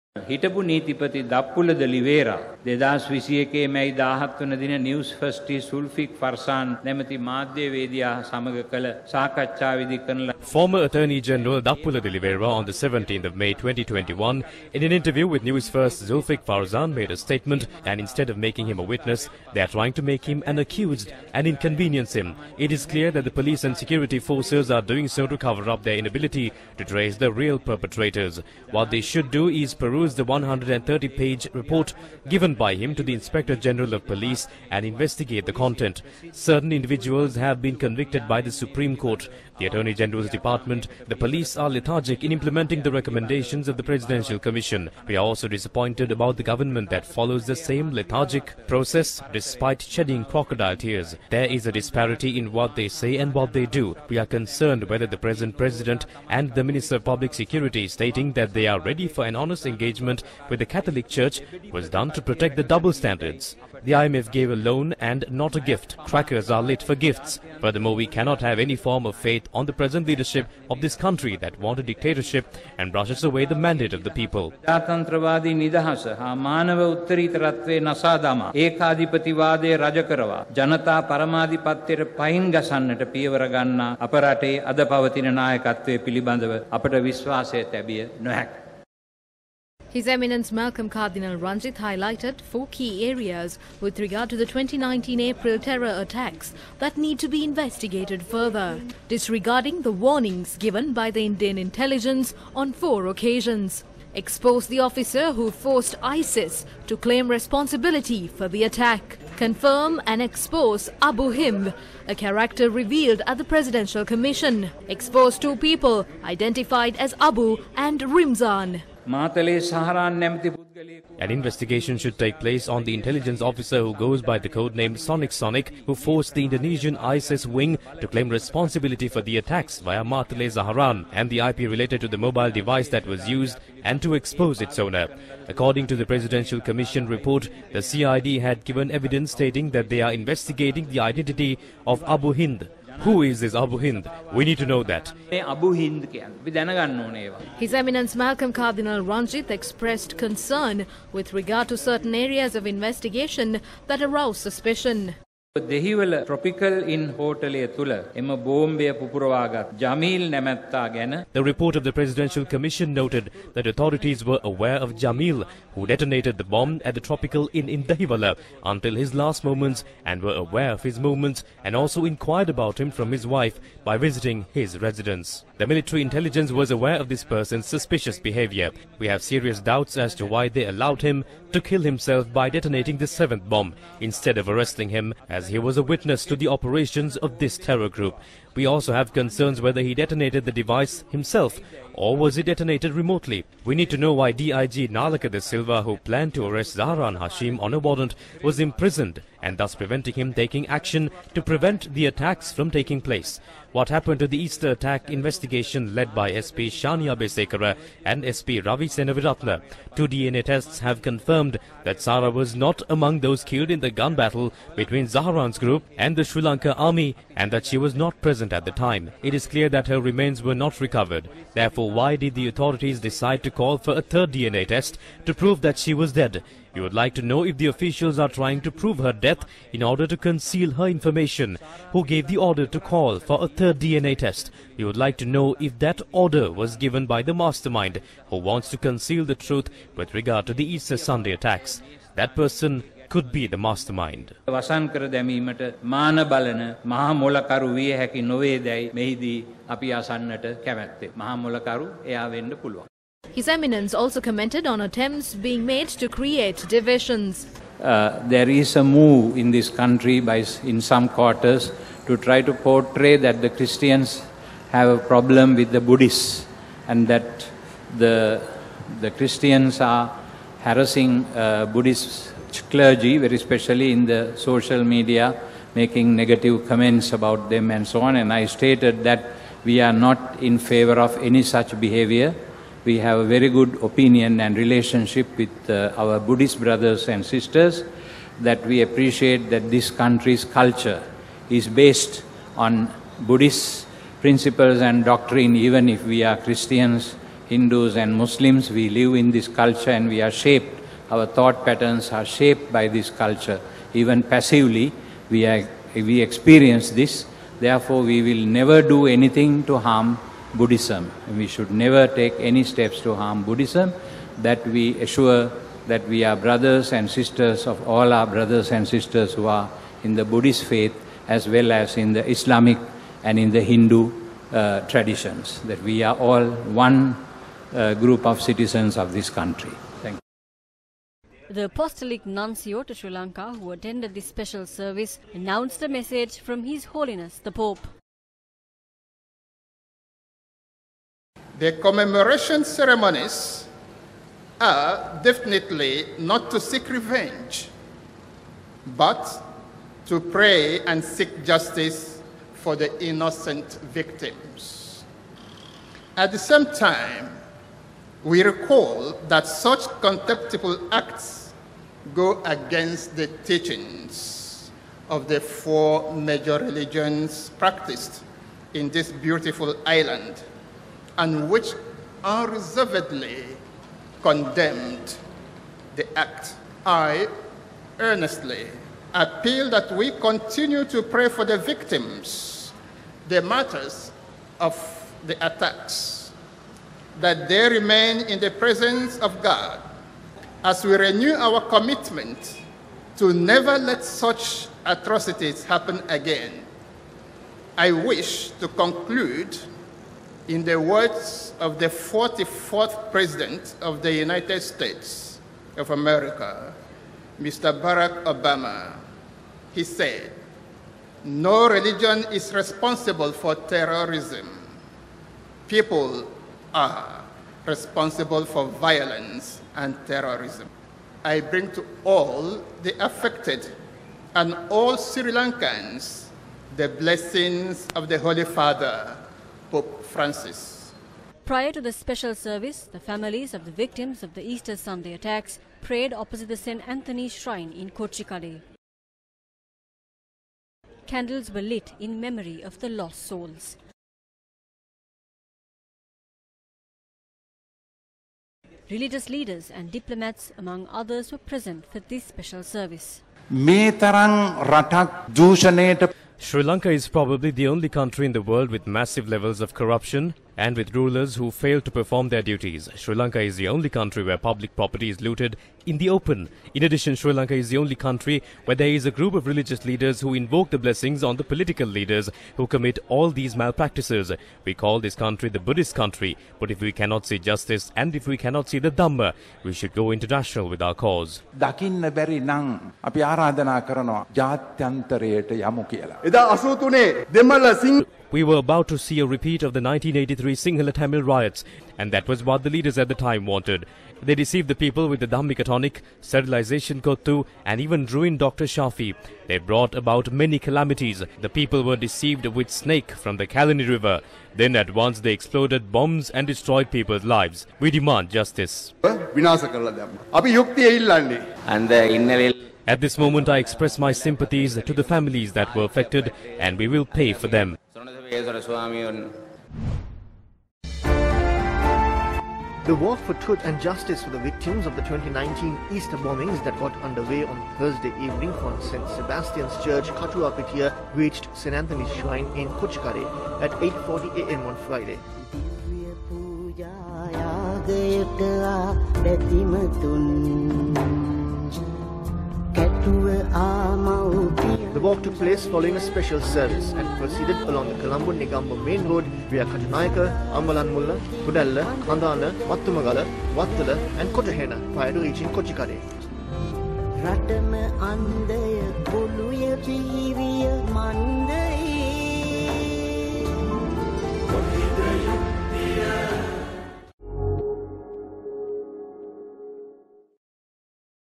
CARDINAL MALCOLM -"පාස්කු ප්‍රහාරයට යුක්තිය ඉටු කිරීමට රටේ නායකත්වය ගැන විශ්වාසයක් නෑ"
"පාස්කු ප්‍රහාරයෙන් විපතට පත් වූවන්ට යුක්තිය ඉටු කිරීමට රටේ නායකත්වය ගැන විශ්වාසයක් නෑ" - අති උතුම් මැල්කම් රංජිත් කාදිනල් හිමිපාණන් වහන්සේ
"No faith in country's leadership in securing justice for Easter Attacks victims"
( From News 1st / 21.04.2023